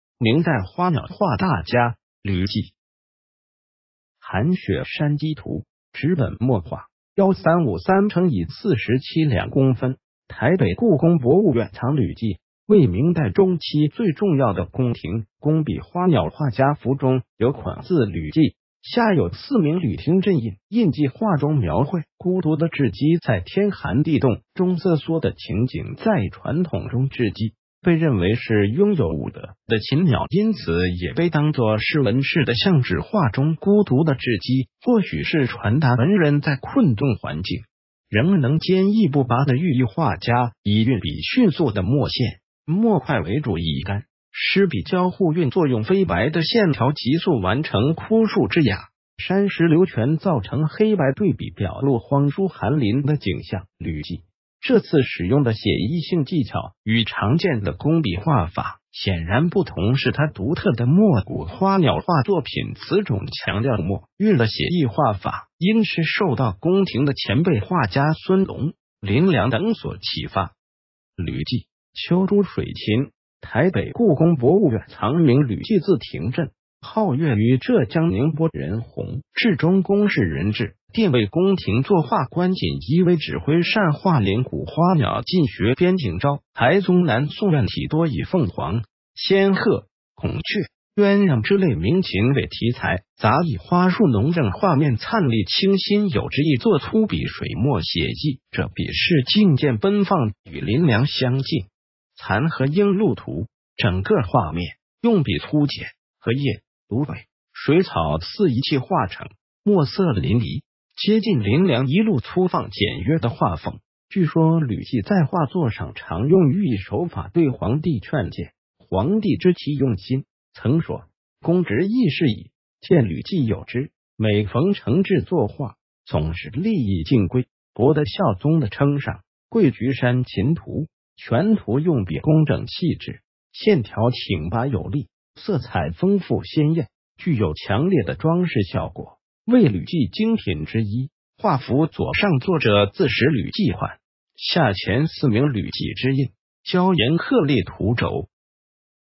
明代花鸟画大家——吕纪
寒雪山鸡图 纸本墨画 135.3×47.2公分 台北故宫博物院藏吕纪为明代中期最重要的宫廷工笔花鸟画家。幅中有款字「吕纪」，下有「四明吕廷振印」印记。画中描绘孤独的稚鸡，在天寒地冻中瑟缩的情景。在传统中，稚鸡被认为是拥有「五德」的禽鸟，因此也被当作是文士的象徵，画中孤独的稚鸡，或许是传达文人在困顿环境，仍能坚毅不拔的寓意。画家以运笔迅速的墨线、墨块为主，以乾、湿笔交互运作，用飞白的线条，疾速完成枯树枝枒、山石流泉，造成黑白对比，表露荒疏寒林的景象。吕纪这次使用的写意性技巧，与常见的工笔画法显然不同，是他独特的「没骨」花鸟画作品。此种强调墨韵的写意画法，应是受到宫廷的前辈画家孙隆、林良等所启发。吕纪 秋诸水禽 台北故宫博物院藏明，吕纪，字廷振，号乐愚，浙江宁波人。弘治中供事仁智殿，为宫廷作画，官锦衣卫指挥。擅画临古花鸟，近学边景昭，还宗南宋院体，多以凤凰、仙鹤、孔雀、鸳鸯之类鸣禽为题材，杂以花树浓盛，画面灿丽清新有致。亦作粗笔水墨写意者，笔势劲健奔放，与林良相近，与边景昭同为明代院体花鸟画中的临古派的代表作家。

▲残荷鹰鹭图，整个画面，用笔粗简，荷叶、芦苇、水草似一气画成，墨色淋漓，接近林良一路粗放简约的画风。据说吕纪在画作上常用寓意手法对皇帝劝谏，皇帝知其用心，曾说：“工执艺事以，谏吕纪有之。”每逢“承制作画”，总是“立意进规”，博得孝宗的称赏。▲桂菊山禽图，全图用笔工整细致，线条挺拔有力，色彩丰富鲜艳，具有强烈的装饰效果，为吕纪精品之一。画幅左上作者自识“吕纪”款，下钤“四明吕纪之印”。▲蕉岩鹤立图轴